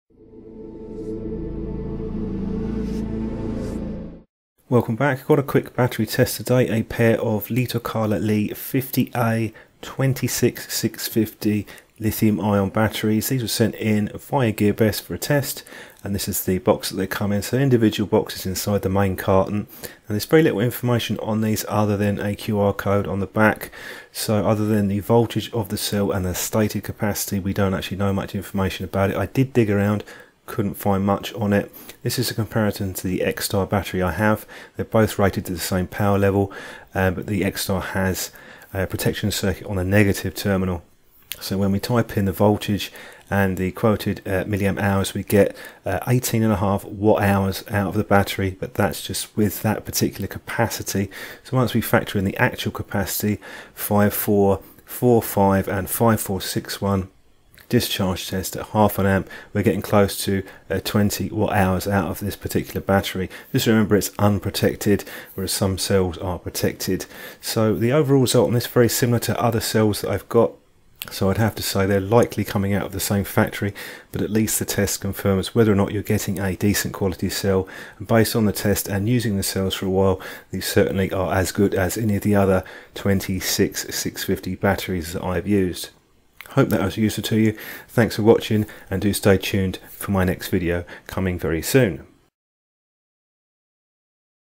LiitoKala Lii - 50A 5000mAh 26650 Batteries: Test
A quick look at the affordable 26650 Li-ion batteries from LiitoKala, they are rated to 5000mAh, but what's the actual capacity of these cells?

Sample supplied via Gearbest for unbiased testing
Details from the maker:
Product Specification:
● Nominal capacity: 5000mAh
● Nominal voltage: 3.7V
● Rechargeable battery: yes
● Rechargeable time: up to 500 times
● Internal resistance: less than 20 milliohm
● Limited charge voltage: 4.2V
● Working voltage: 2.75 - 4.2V